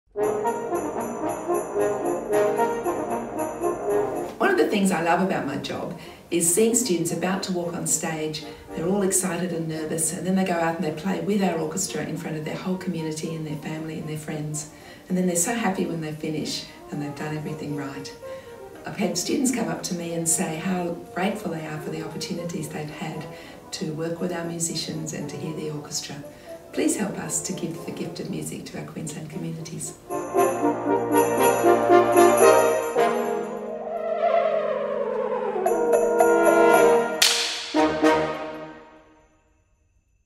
Christmas Appeal 2019 | Judy Wood, Community Engagement Manager
Help us give the Gift of Music to our Queensland communities. Please give generously this Christmas.